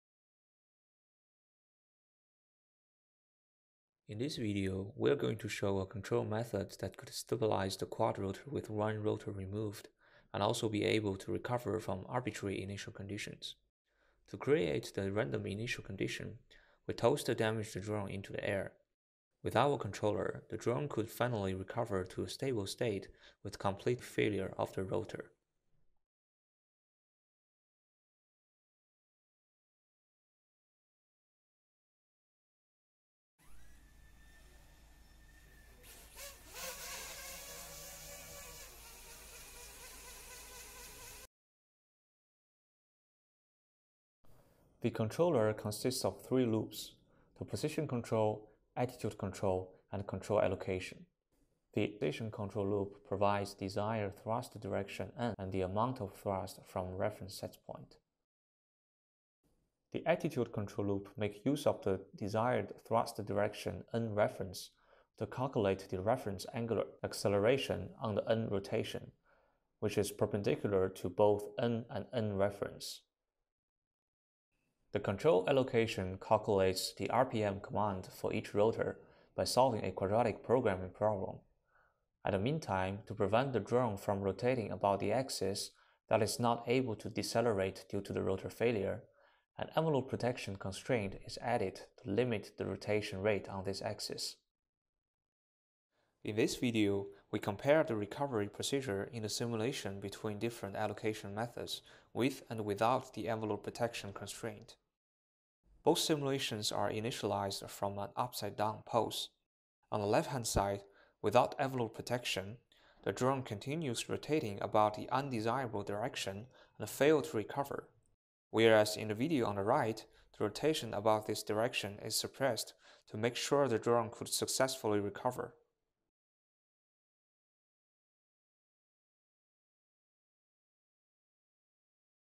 Quadrotor Upset Recovery after Complete Loss of a rotor
This work has been published in
Sun, S., Baert, M., van Schijndel, B.A.S. and de Visser, C., 2020. Upset Recovery Control for Quadrotors Subjected to a Complete Rotor Failure from Large Initial Disturbances. ICRA 2020.